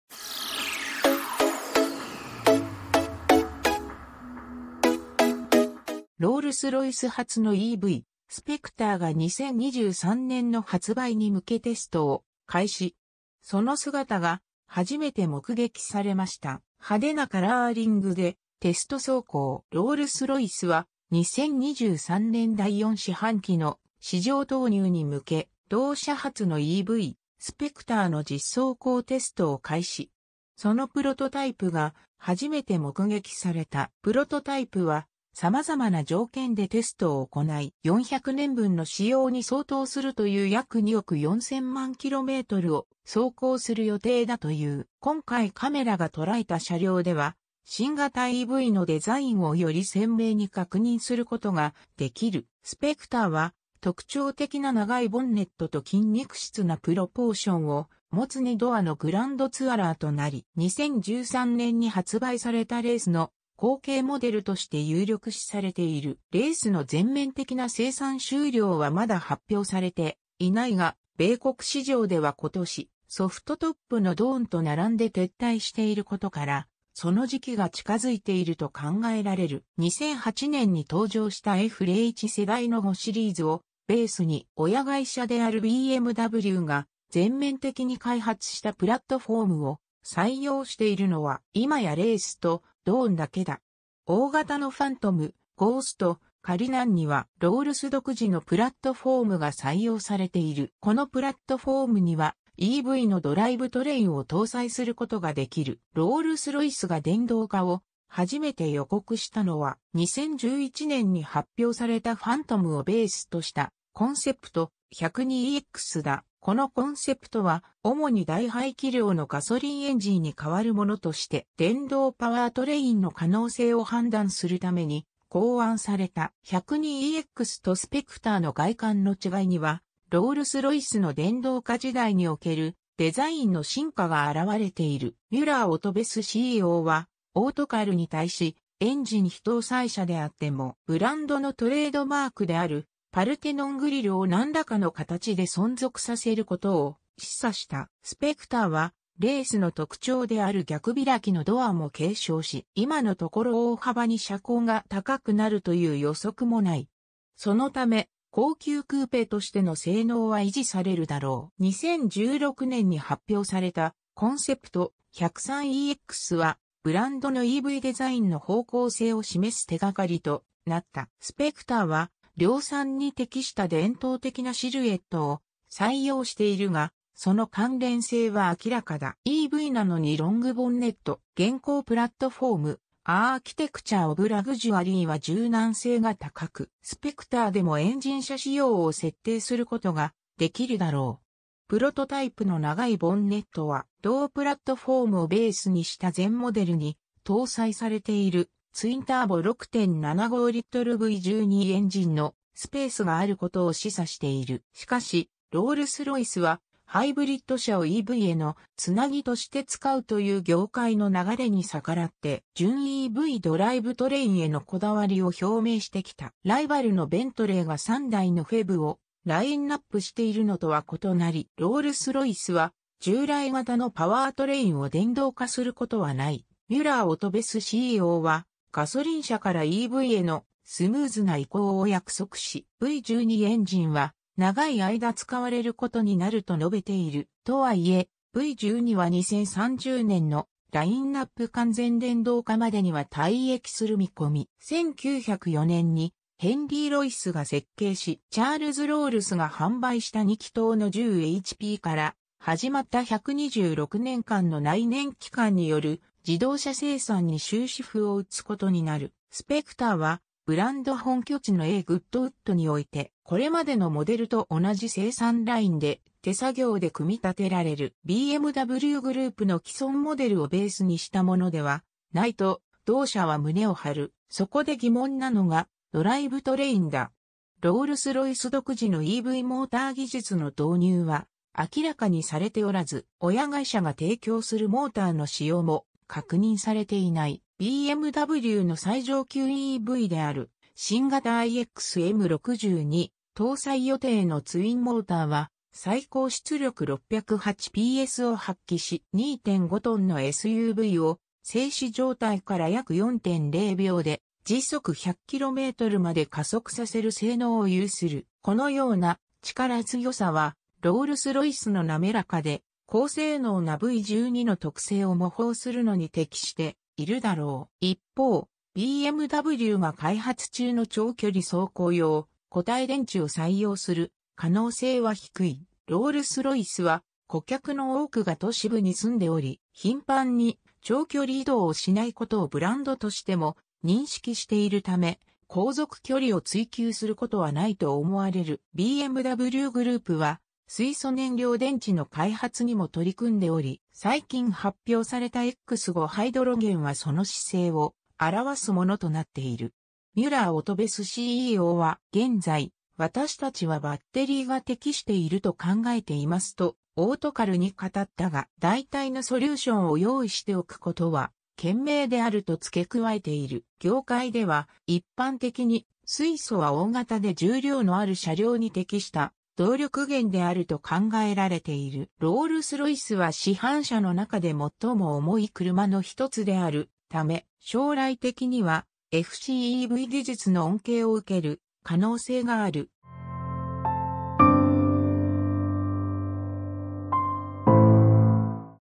新型ロールス・ロイス・スペクター プロトタイプ発見 「レイス」後継となり得るか | 車の話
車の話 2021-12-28

 ロールス・ロイス初のEV、スペクターが2023年の発売に向けテストを開始。その姿が初めて目撃されました。  派手なカラーリングでテスト走行  ロールス・ロイスは、2023年第4四半期の市場投入に向け、  同社初のEV「スペクター」の実走行テストを開始。そのプロトタイプが初めて目撃された。  プロトタイプはさまざまな条件でテストを行い、  400年分の使用に相当するという約2億4000万kmを走行する予定だという。  今回カメラが捉えた車両では、新型EVのデザインをより鮮明に確認することができる。  スペクターは、特徴的な長いボンネットと筋肉質なプロポーションを持つ2ドアのグランドツアラーとなり、  2013年に発売されたレイスの後継モデルとして有力視されている。  レイスの全面的な生産終了はまだ発表されていな